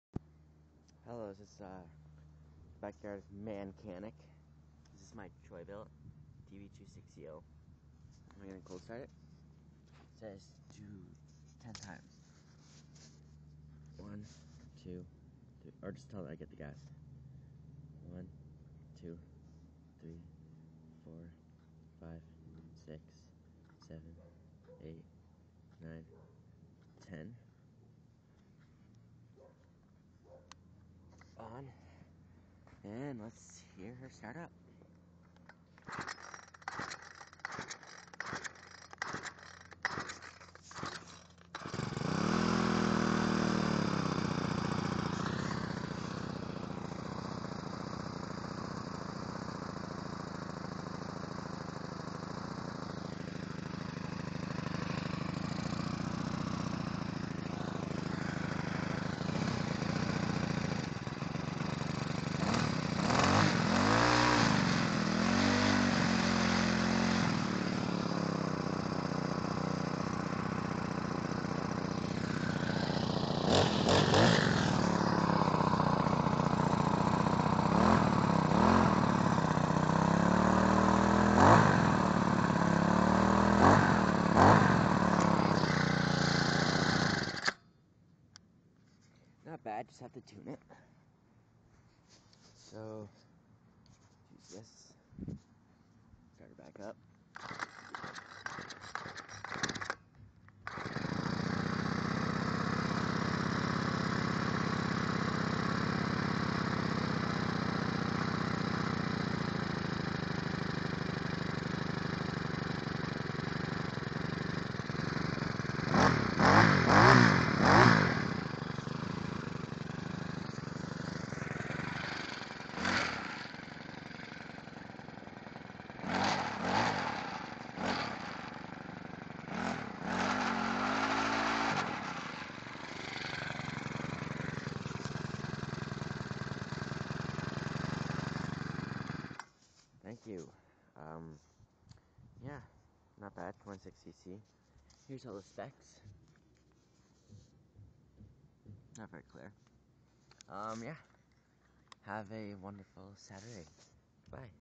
troybilt first start
cold starting my troy first start since last year